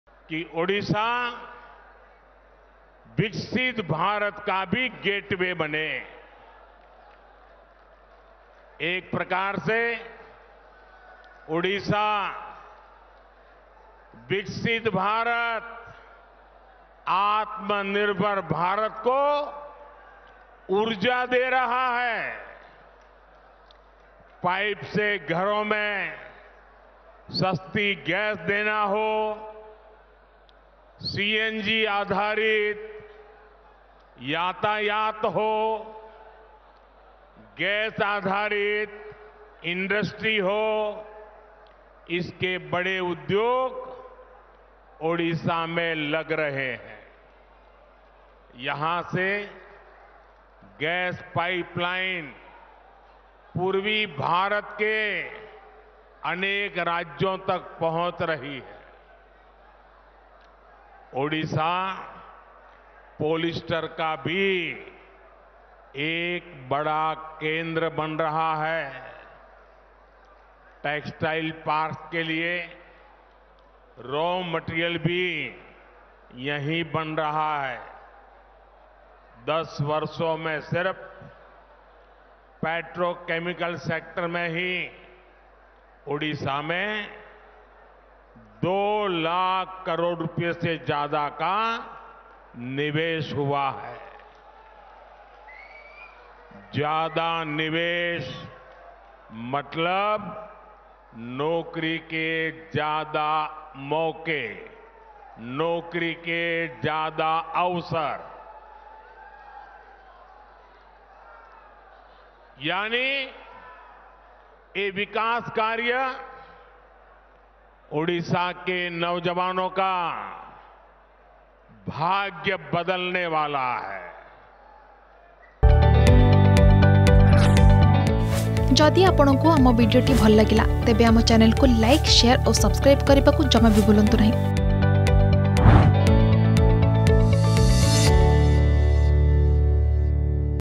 ବିକଶିତ ଭାରତର ଗେଟ ଓ୍ବେ ବନିବ ଓଡିଶା || PM Narendra Modi || Jajpur
ବିକଶିତ ଭାରତର ଗେଟ ଓ୍ବେ ବନିବ ଓଡିଶା : ନରେନ୍ଦ୍ର ମୋଦି,ପ୍ରଧାନମନ୍ତ୍ରୀ